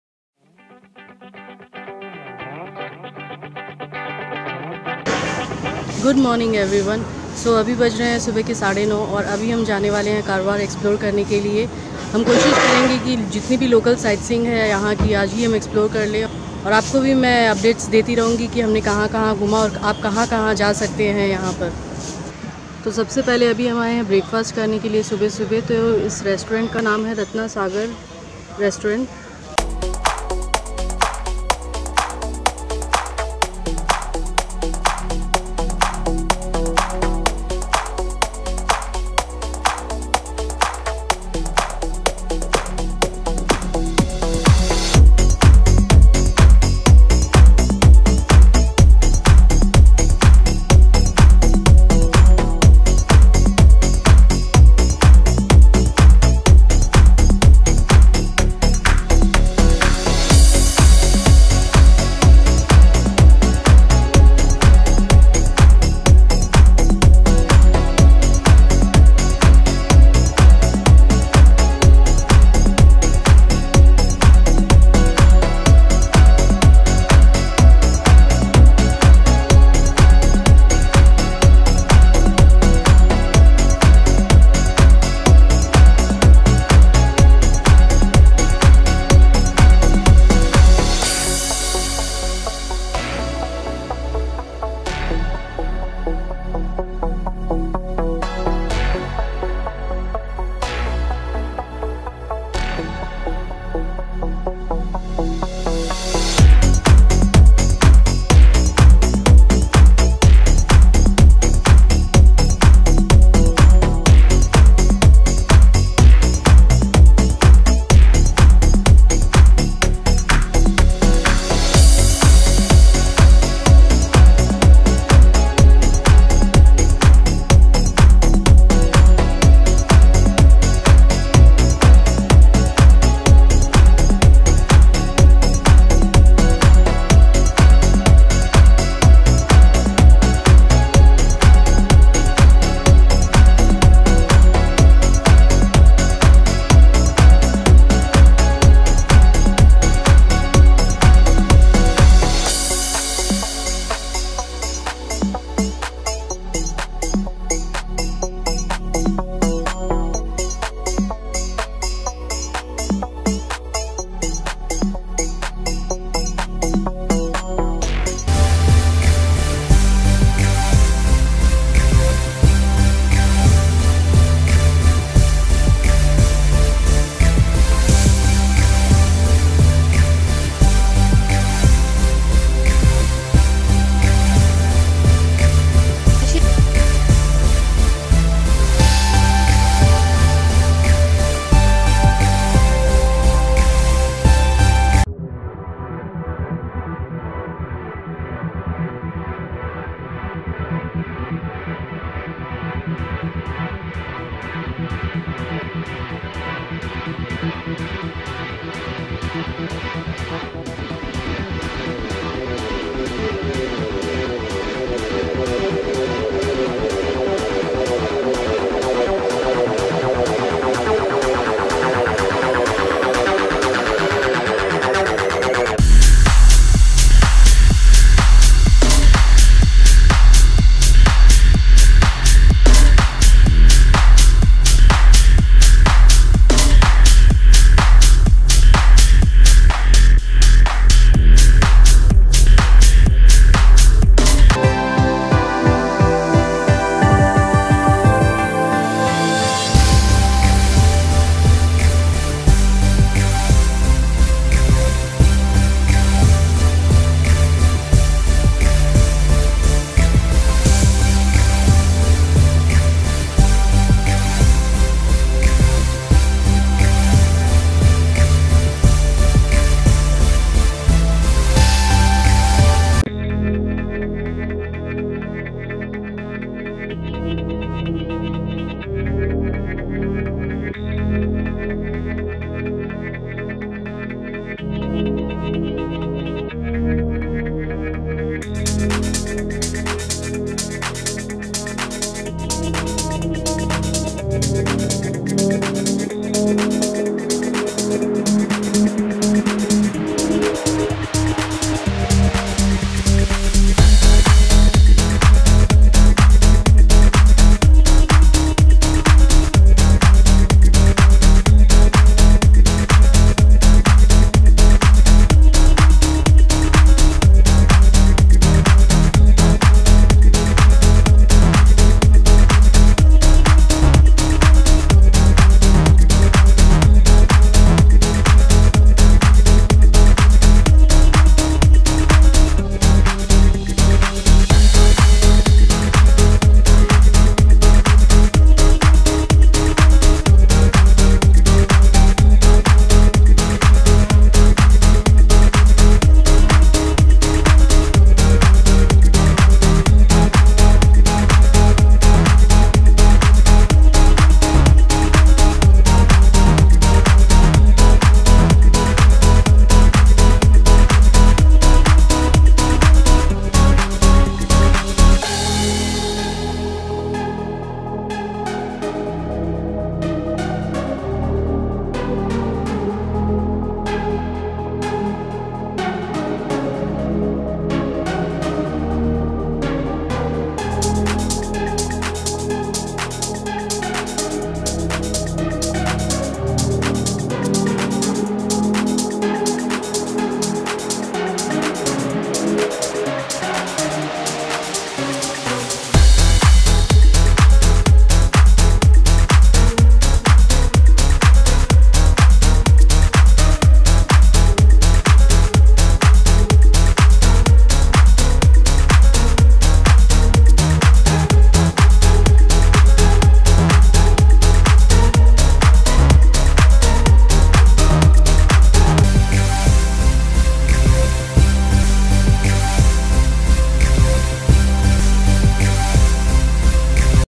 KARWAR |Places to visit in karwar| PART 2 | BY Jaya Sharma
Karwar|Places To visit in Karwar|kali river,tilmati beach,warship museum
Karwar (formerly Carwar) is a city in Uttara Kannada district in the South Indian state of Karnataka and the administrative centre of Uttara Kannada district. ... It is situated between Sahyadri ever green forest in east, blue Arabian Sea to the west, towards south ends with harbour and North the beautiful Kali river.